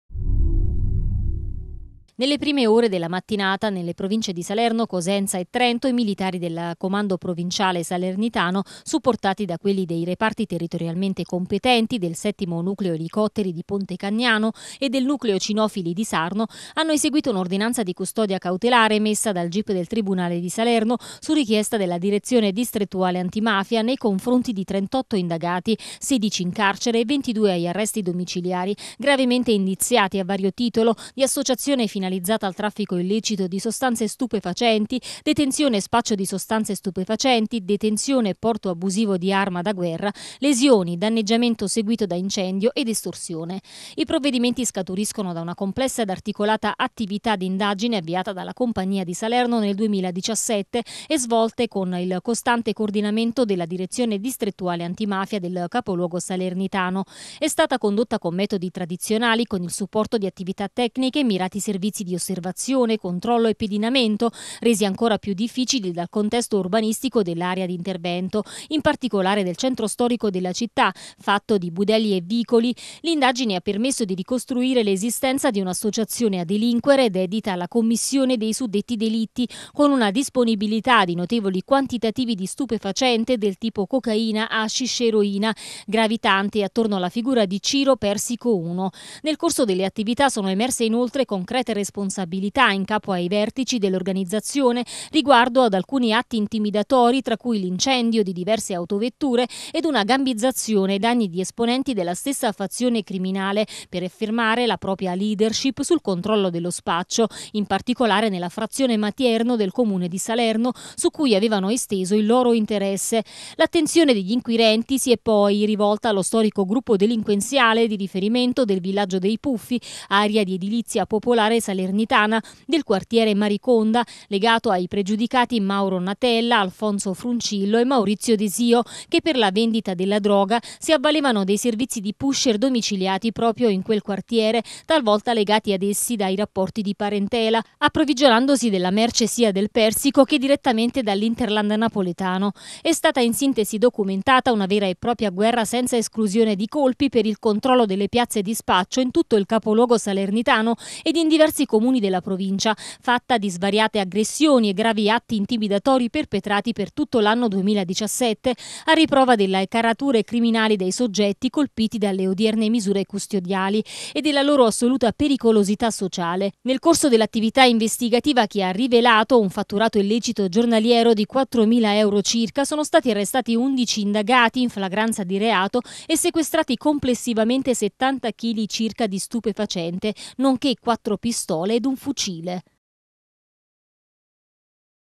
Salerno Arrestate 38 persone per traffico e spaccio di droga ed armi.
Nelle prime ore della mattina, nelle province di Salerno, Cosenza e Trento, militari di questo Comando Provinciale, supportati da quelli dei reparti territorialmente competenti, del 7° Nucleo Elicotteri di Pontecagnano (SA) e del Nucleo Cinofili di Sarno (SA), hanno eseguito un’ordinanza di custodia cautelare – emessa dal GIP del Tribunale di Salerno, su richiesta della Direzione Distrettuale Antimafia – nei confronti di 38 indagati (16 in carcere e 22 agli arresti domiciliari).